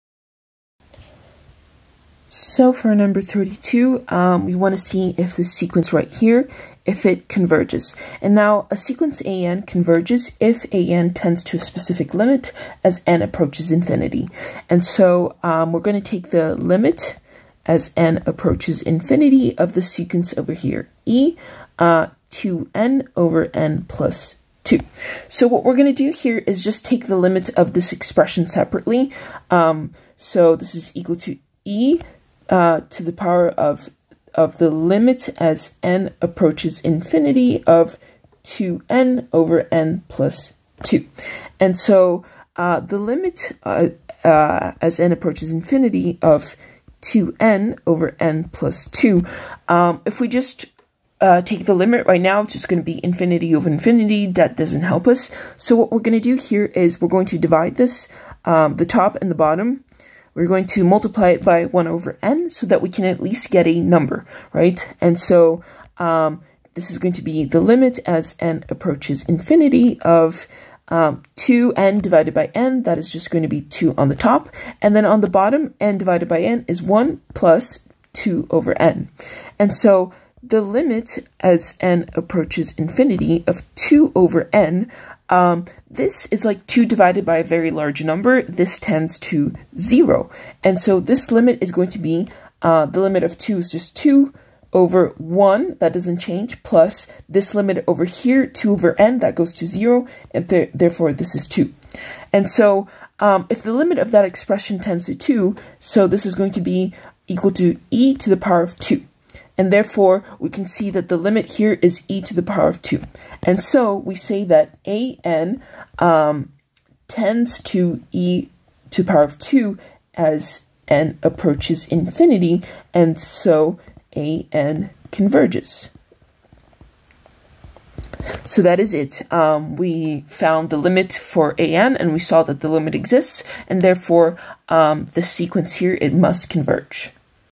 11.1.32 - Determine whether the sequence converges or diverges. Find the limit. an = e^(2n/(n+2))
Problem 11.1.32 From James Stewart's Single Variable Calculus - Early Transcendentals 7th edition from chapter 11, Inifinite Sequences and Series -  Sequences.

Determine whether the sequence converges or diverges. If it converges, find the limit. an = e^(2n/(n+2))